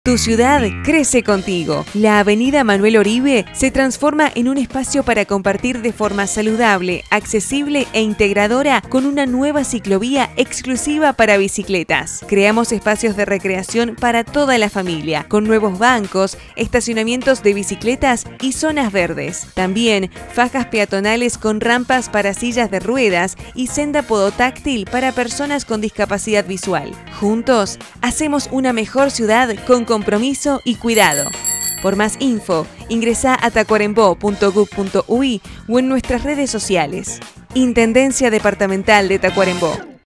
Ciclovía Avenida Manuel Oribe. Spot Publicitario.
Campaña "Tu ciudad crece contigo". Ciclovía en Avenida Manuel Oribe. Spot publicitario 01.